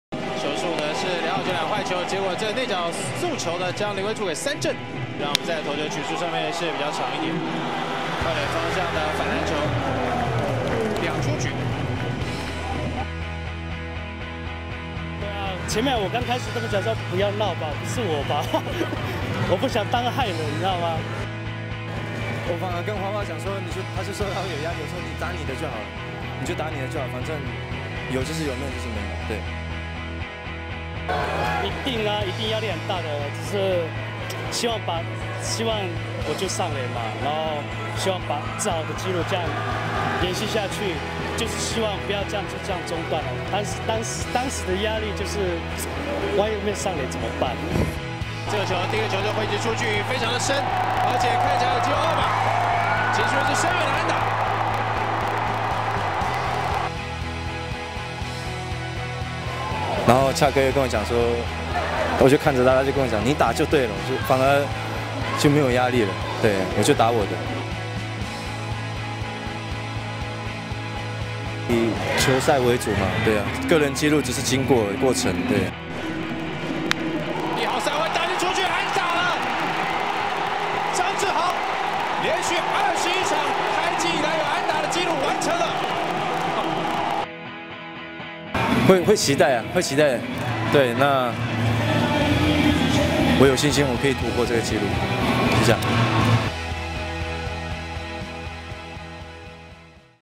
【CPBL2016】張志豪挑戰開季連續安打紀錄心情
張志豪九局上半挑戰開季連續21場安打紀錄的心情
讓他自己說給你聽
張志豪：『謝謝張正偉的助攻，還有恰恰彭政閔的悄悄話…..』